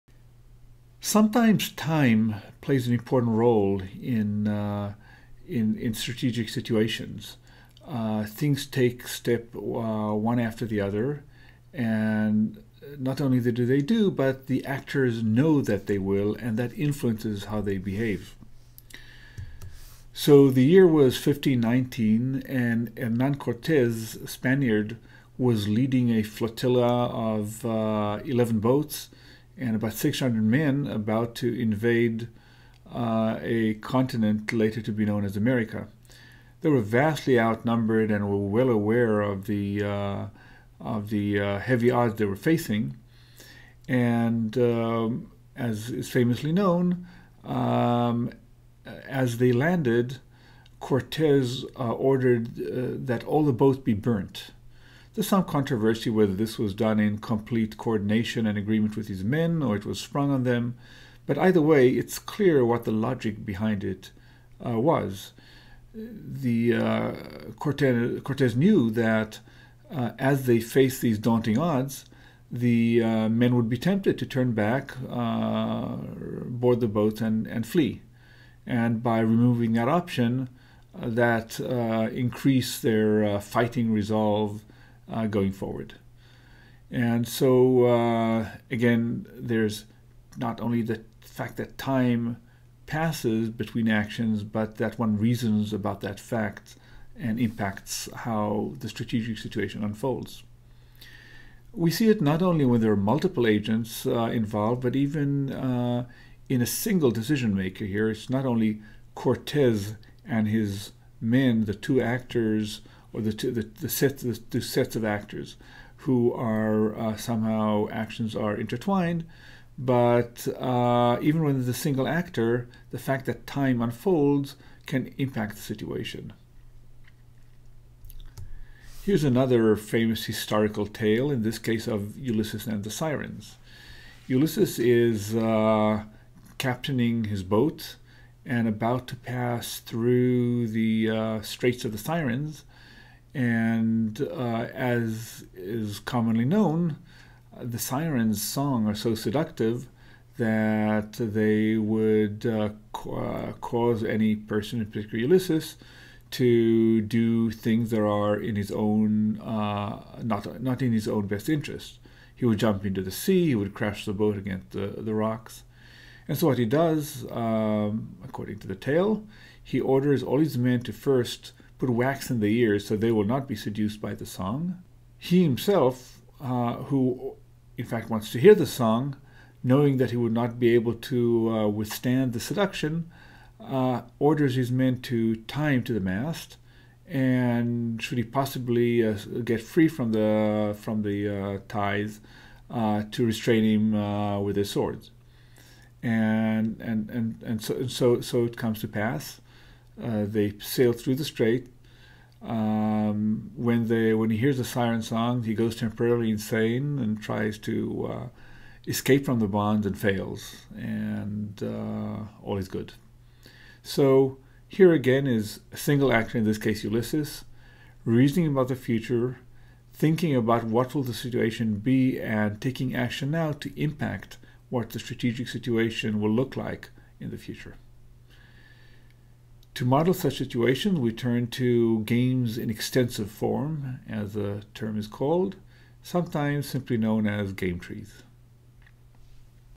GTO-4-01: Perfect Information Extensive Form: Taste
introduces the notion of acting now to affect the strategic situation in the future, which will be formalized in later videos as extensive form games (or game trees).  It features Yoav Shoham (Stanford).